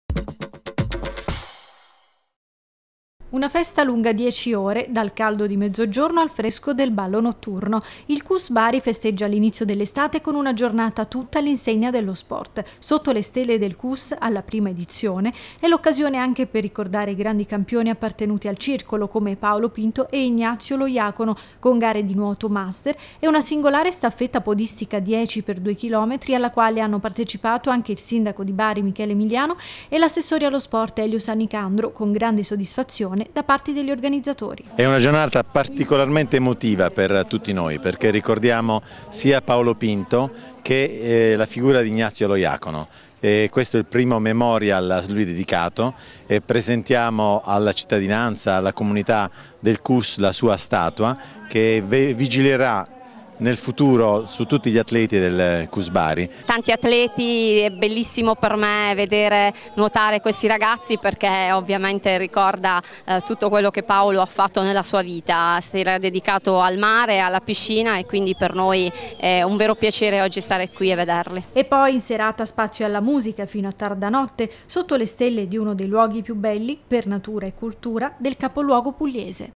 TG 11.07.11 Una festa d'estate lunga un giorno per il Cus bari
Lo sport e l'allegria insieme per una giornata al Cus Bari, che ha visto protagonisti atleti e non atleti nella prima edizione di "Sotto le stelle del Cus.